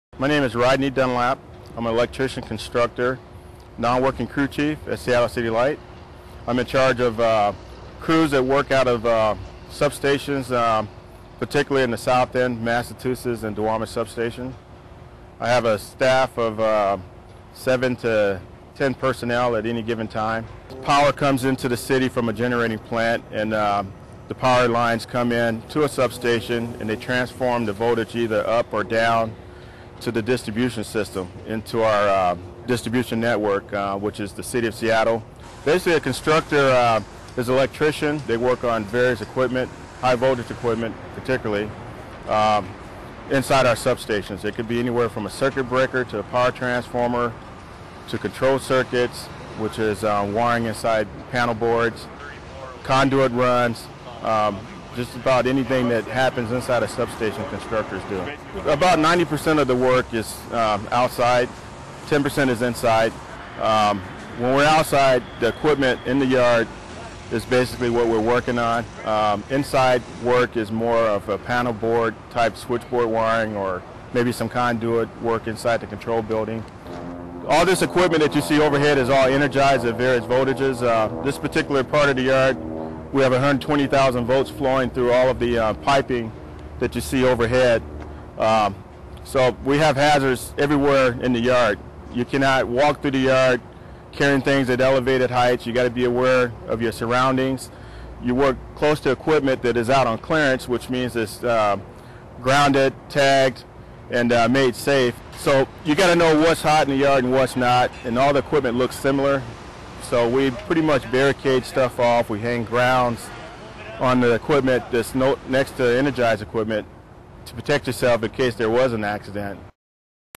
Electrician Constructors -- Bringing the Power to Your Homes (closed caption)
Rodney Dunlap is an electrician constructor supervisor, who oversees operations at two major substations for City Light. Constructors are working around very high voltages and are very conscious of their surroundings at all times.

• Información en español
• Impormasyon sa Tagalog
• 中文資訊
• Thông tin bằng tiếng Việt
• 한국어 정보
• Macluumaad Af-Soomaali ah